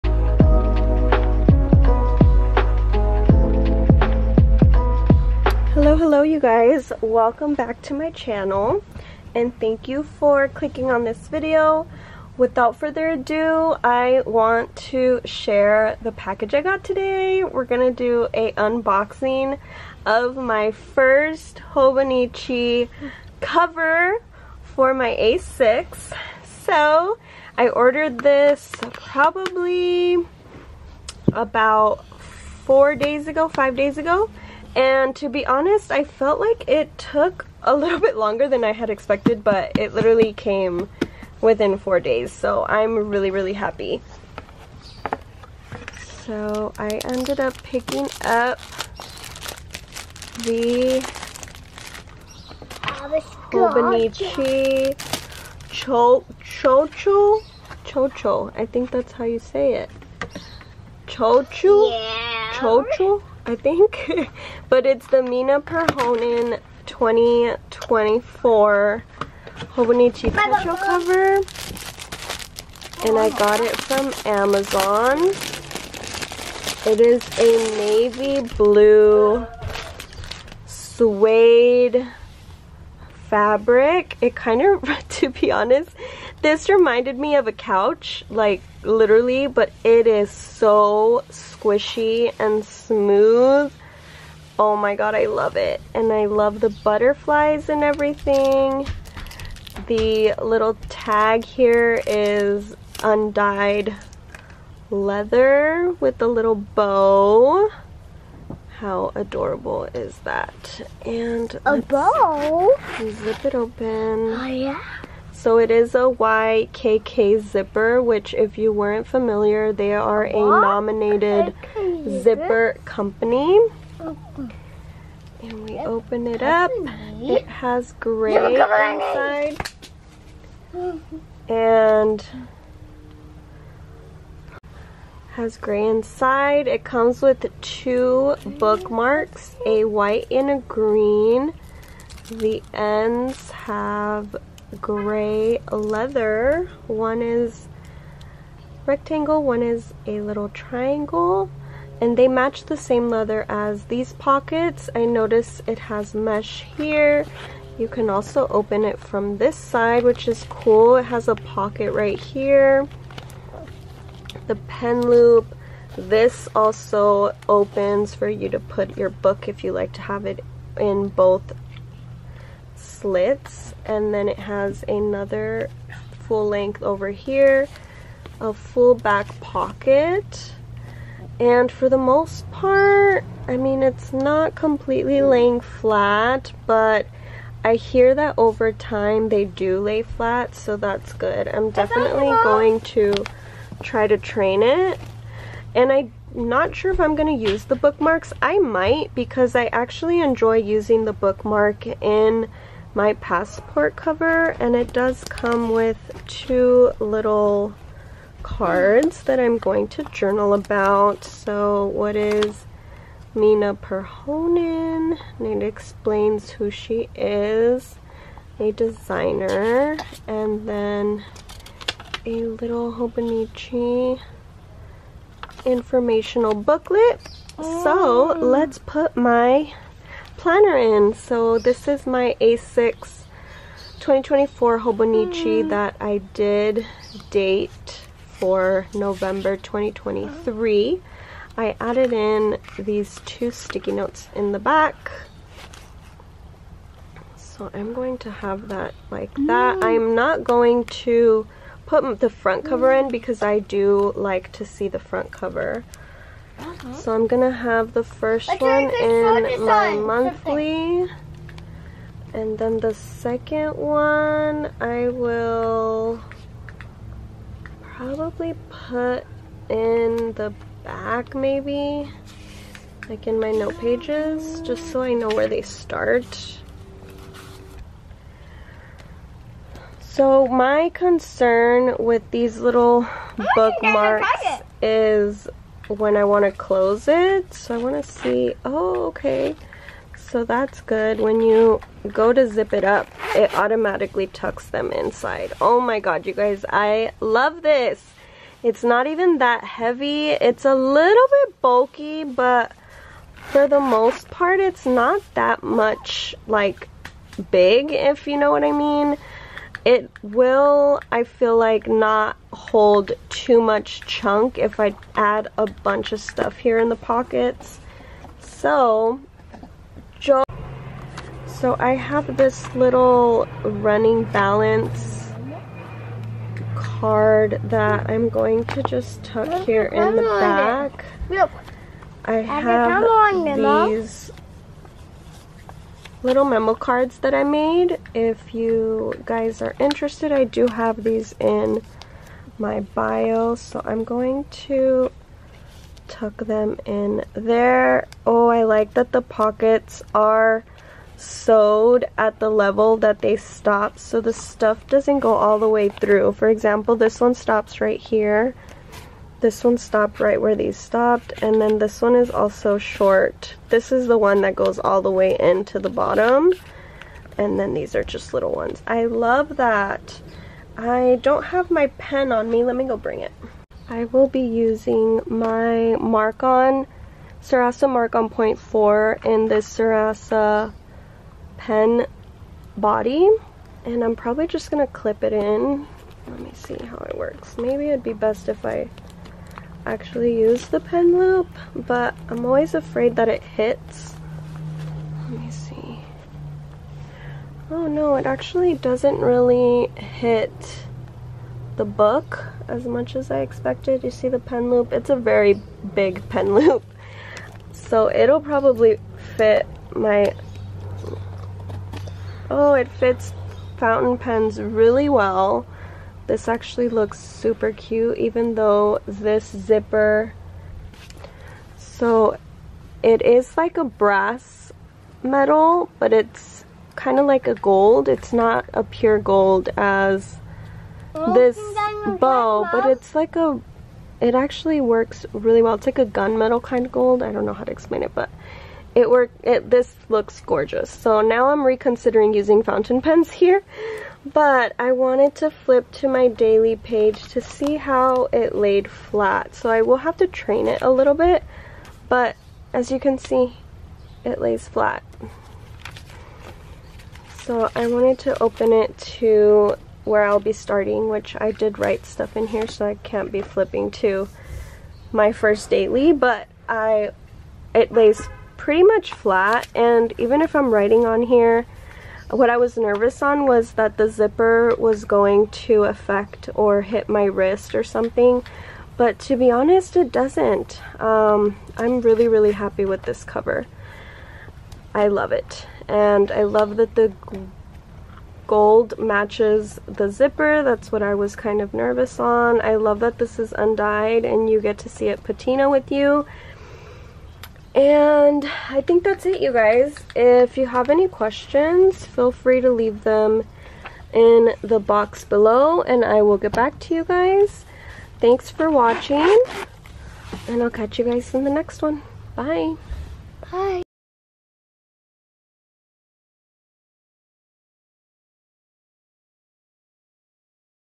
UNBOXING | HOBONICHI MINA PERHONEN // CHOUCHU COVER A6 ZIP
Hi there! In this video I have an unboxing of my new 2024 Hobonichi Techo. I also chat a bit about my plans and decorate my front cover.

⟡ ITEMS MENTIONED/SHOWN:
Hobonichi A6 Cover
Zebra Sarasa Grand

⟡ SOCIALS:
Planner IG: @writtentreasuries
Amazon Storefront

All items were purchased with my own money. I appreciate your support so much!